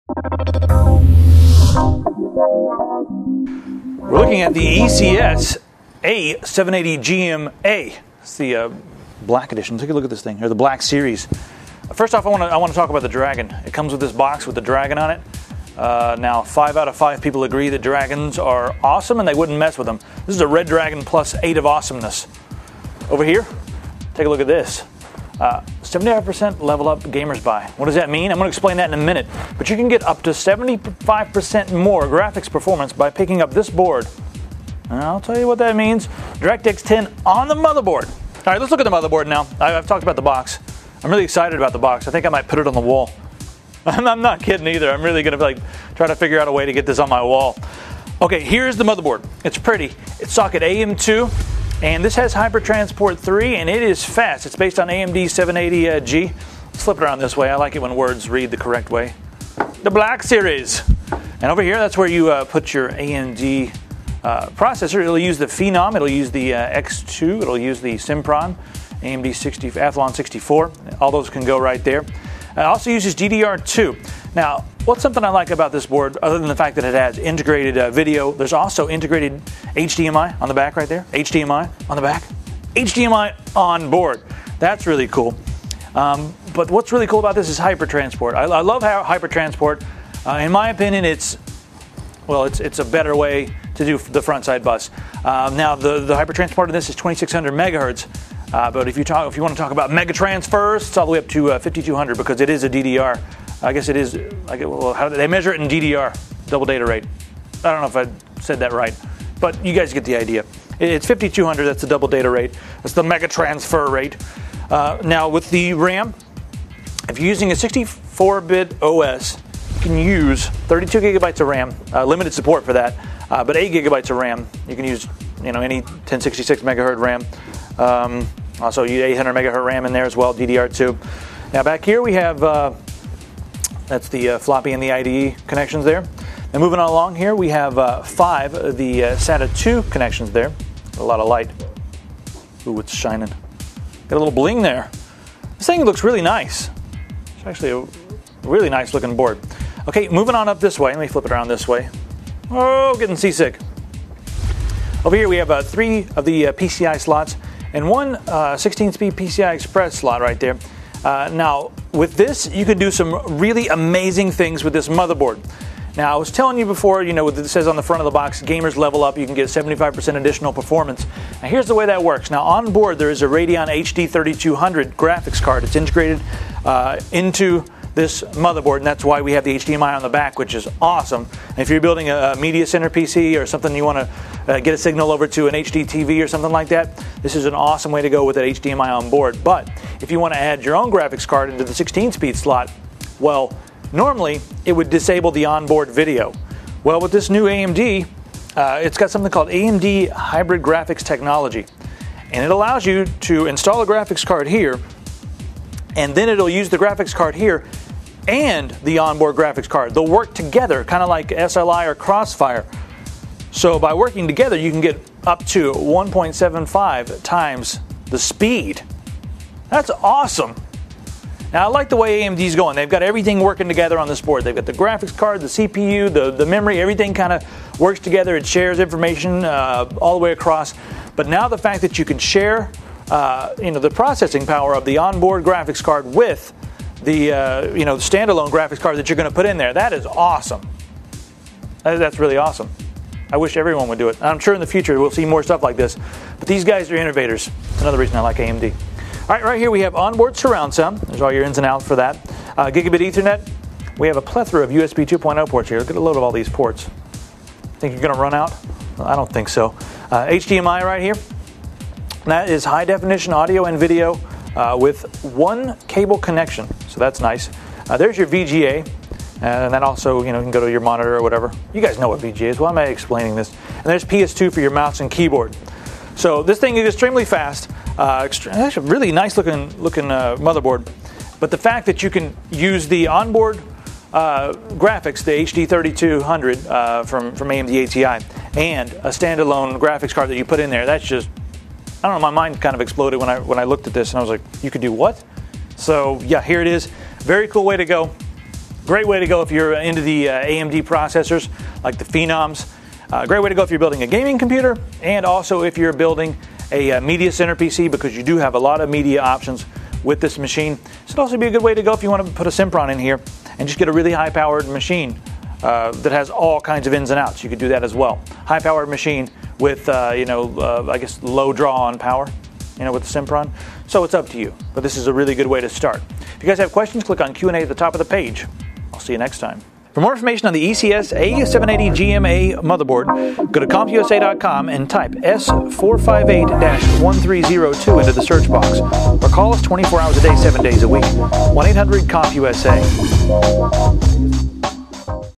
ECS A780GM-A Motherboard
Get superb performance at an affordable price with our new ECS A780GM-A (V1.0) AMD 780G Socket AM2+ ATX motherboard. With its ATI Radeon HD3200 and Integrated Direct X10 graphics support this platform delivers spectacular video for systems running on the high-powered AMD Phenom, AMD Athlon 64X2 dual core, Athlon 64, and Sempron processors. The ECS A780GM-A supports Hyper Transport 3.0 CPU interface, dual-channel 1066/800/667/533/400 DDR2 memory, PCI Express x16/x1, SATA 3.0Gb/s, RAID 0, 1, 10 configurations, 8-channel HD audio, USB 2.0, SPDIF, HDMI, and Gigabit Fast Ethernet LAN.